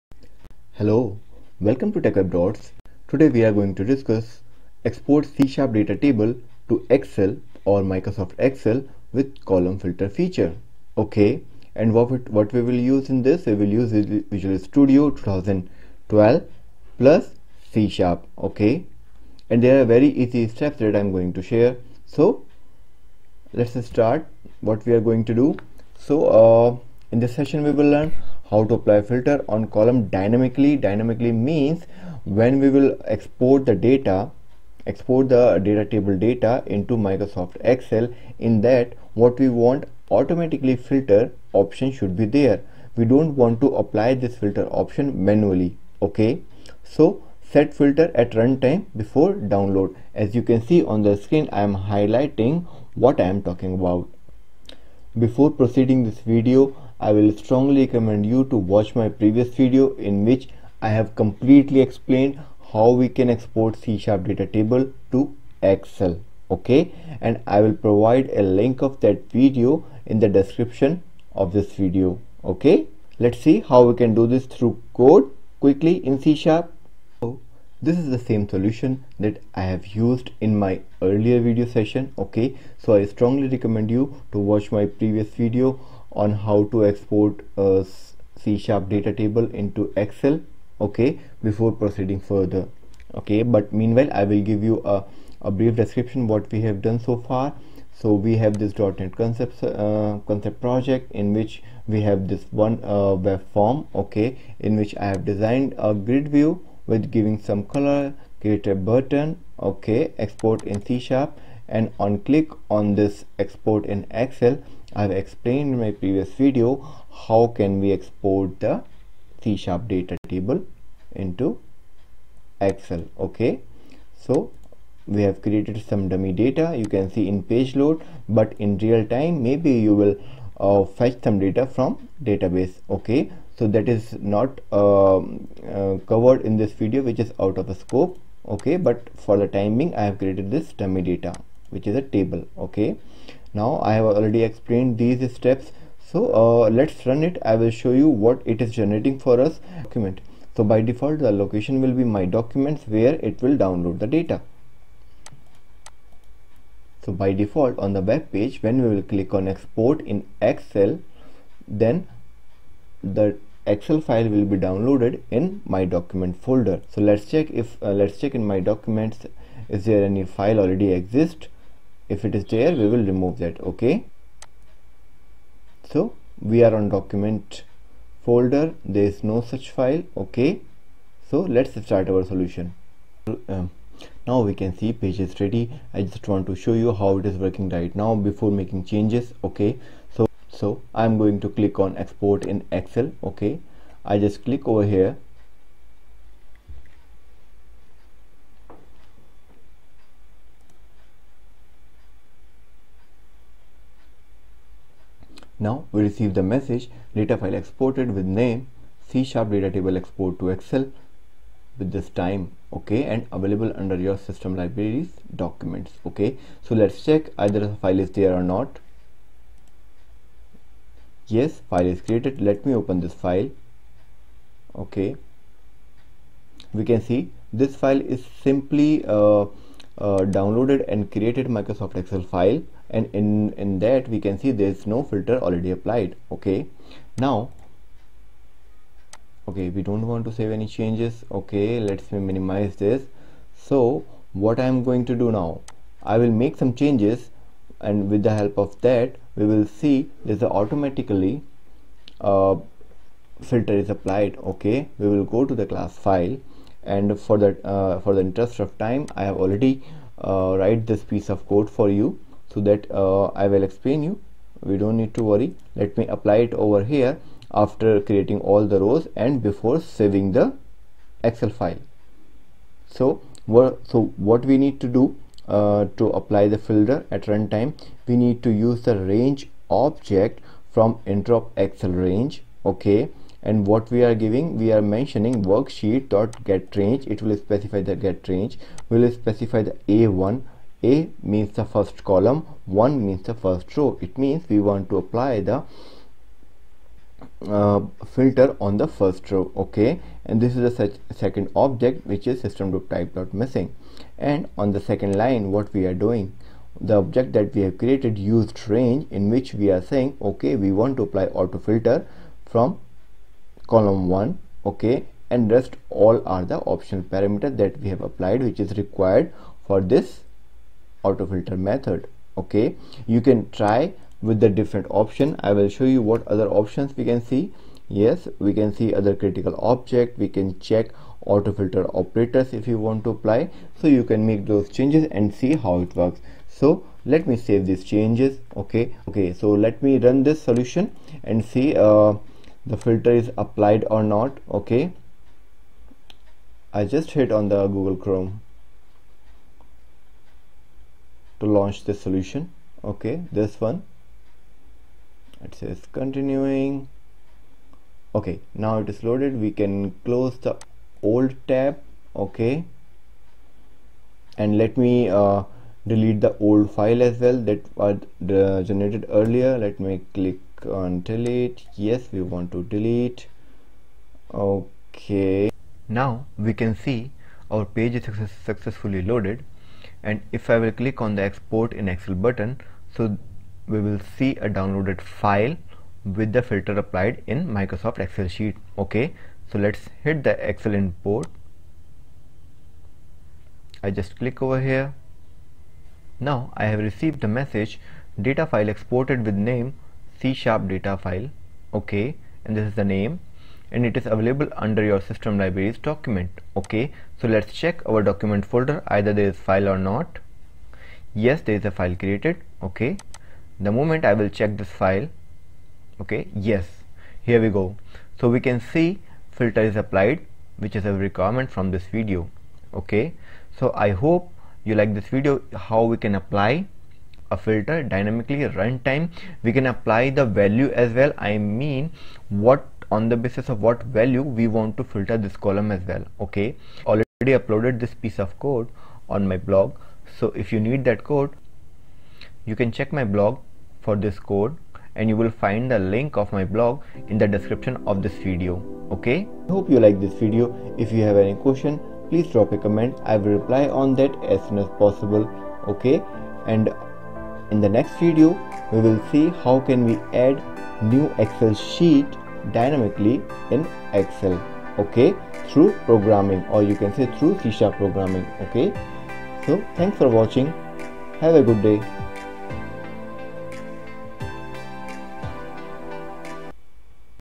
How to convert C# DataTable into Excel with column filter | ASP.NET
I have explained the followingin this:
How to Export/convert C# DataTable into Excel with Column Filter
Export ASP Net GridView Data into Excel with Column Filter